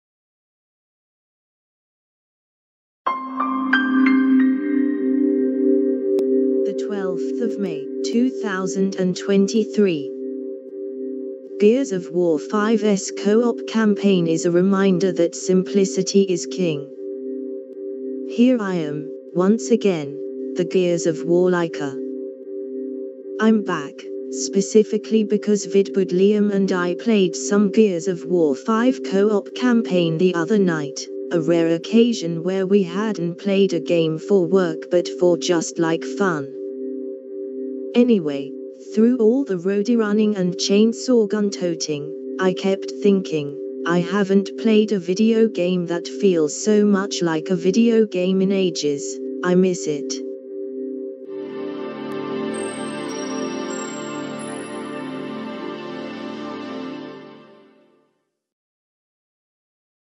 [Latest]Gears Of War 5's co-op campaign is a reminder that simplicity is king
[Contents]
Here I am, once again, The Gears Of War-liker. I'm back, specifically because vidbud Liam and I played some Gears Of War 5 co-op campaign the other night, a rare occasion where we hadn't played a game for work but for just like fun? Anyway, through all the roadie-running and chainsaw-gun-toting, I kept thinking, "I haven't played a video game that feels so much like a video game in ages, I miss it".